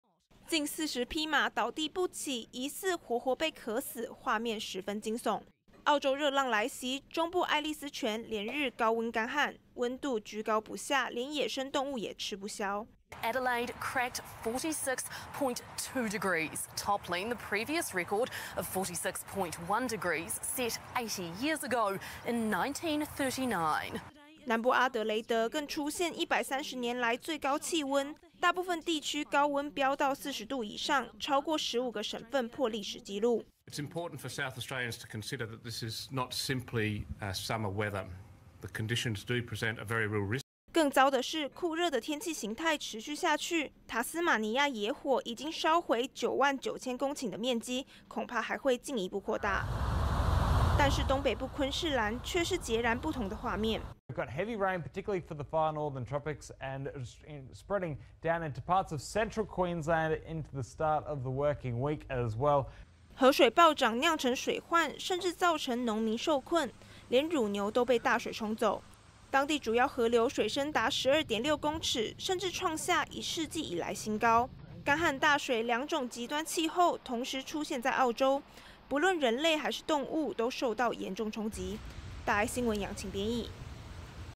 澳洲酷暑百年高溫 昆士蘭卻豪雨成災
澳洲今年氣候格外異常，東北部、昆士蘭地區豪雨成災，當地一條主要河流更出現118年來，最高水位。而南部地區卻是酷暑的天氣，氣溫飆到46.2度，創130年來高溫，中部地區更出現嚴重乾旱。政府單位憂心、如果這波熱浪持續，恐怕擴大原先林火面積，造成更多災情。